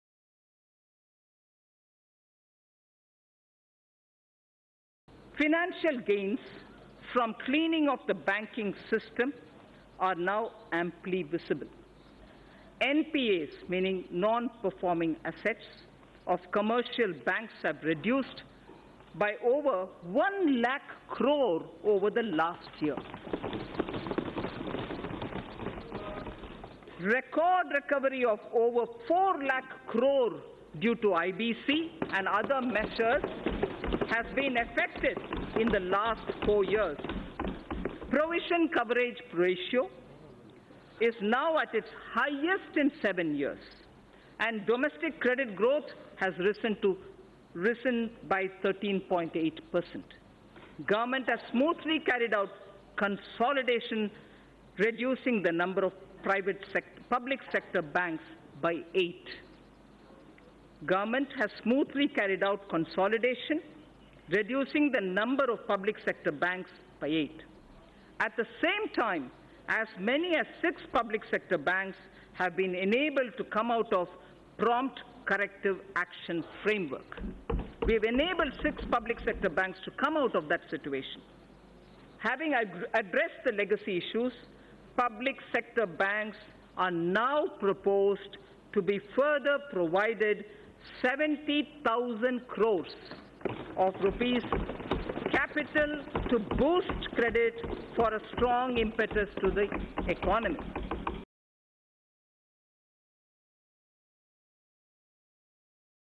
Budget 2019: NPAs worth Rs 4 lakh crore recovered in past 4 years, says Nirmala Sitharaman
New Delhi, July 05 (ANI): Union Finance Minister Nirmala Sitharaman presented the budget for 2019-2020 in the national capital today.